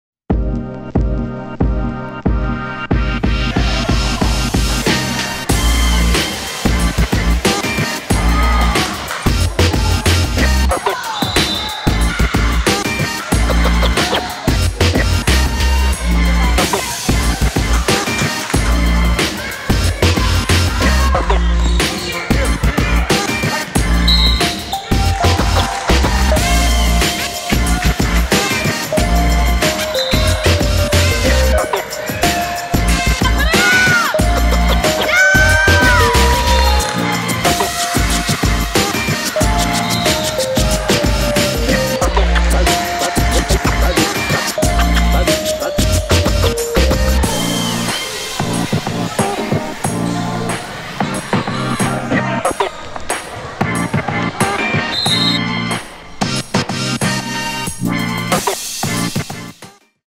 AlwaysLive Girls AAU State Championship Saturday Morning Highlights
Highlights from some of the action at the AlwaysLive Girls AAU State Championship in Mooresville, NC!